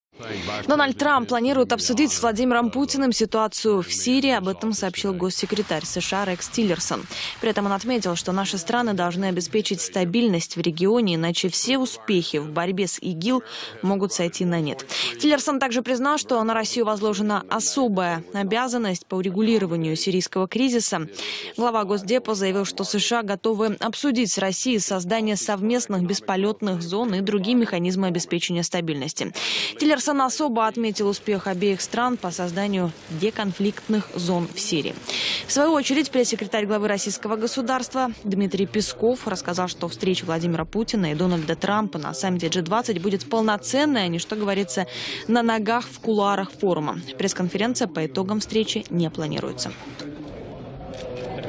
Дональд Трамп хочет обсудить на встрече с Владимиром Путиным ситуацию в Сирии
Глава Госдепа признал особую роль России в урегулировании сирийского конфликта